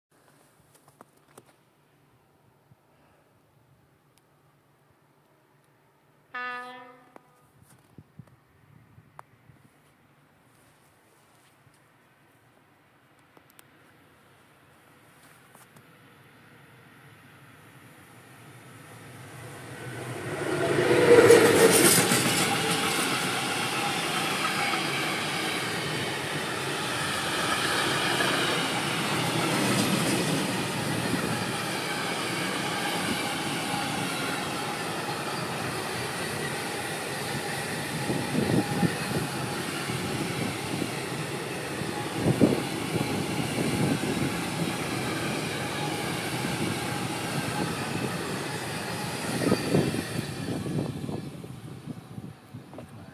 [ECCO Rail] 182 118-0 | Wilkowo Świebodzińskie
Skoda ze składem kontenerów przejazdem przez p.o. Wilkowo Świebodzińskie w kierunku Rzepina, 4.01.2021